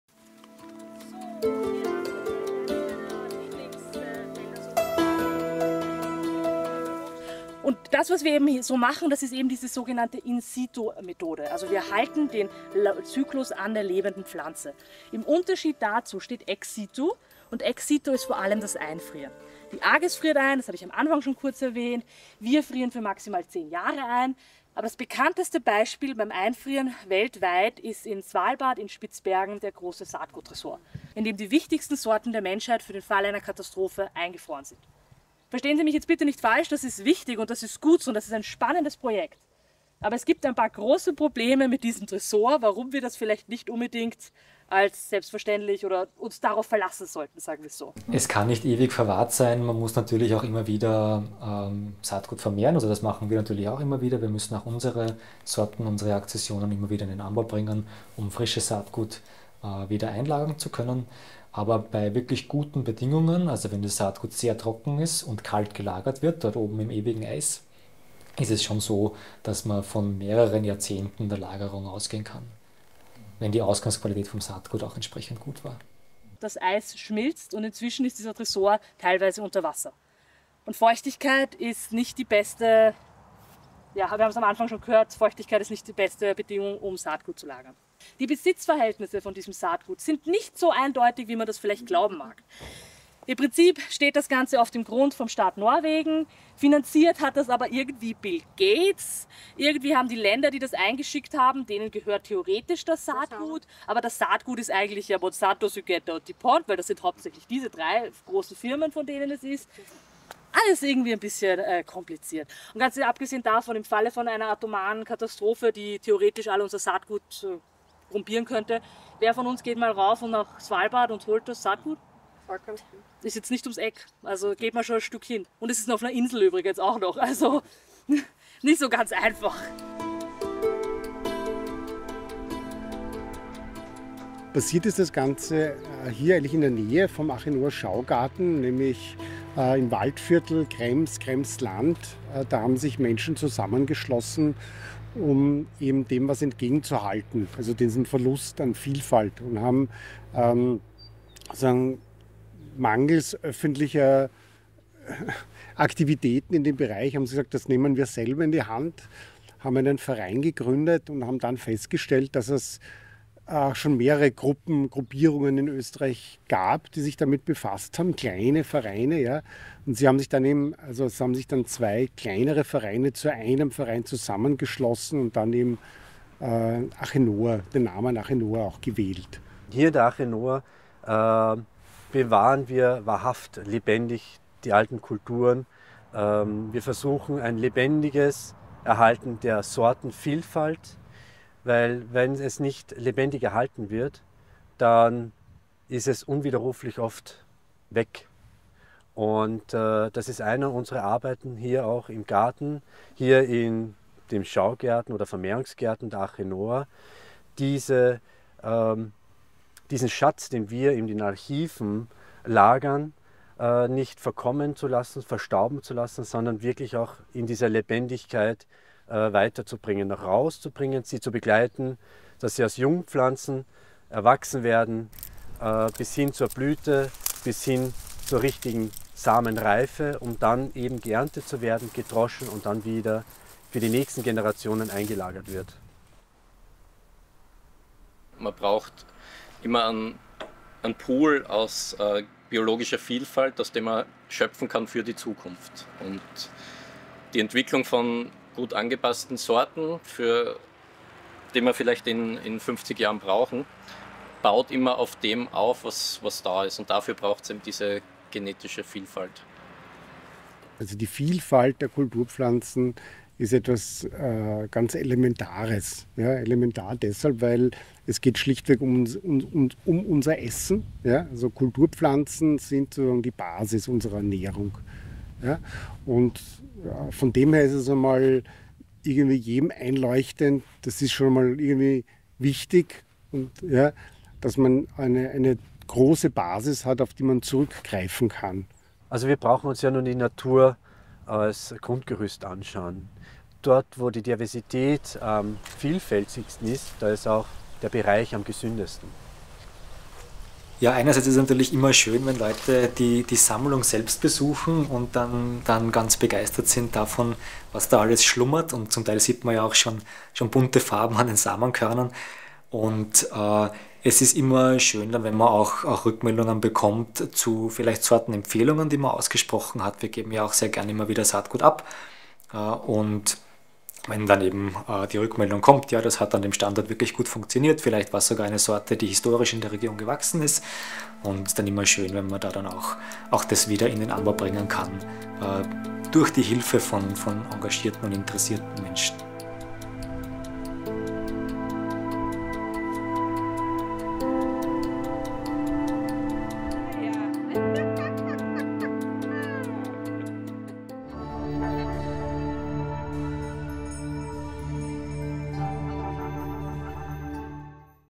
Spitzbergen vs ArcheNoah Saatgutspeicher
Gibt es ein Problem mit dem Saatgutspeicher in Spitzbergen? Auf jeden Fall gibt es eine Menge unbequemer Fragen zu diesem Thema.
Der Saatgutspeicher in Österreich namens Arche Noah hat wiederrum eigene Philosophie, die hier beleuchtet wird.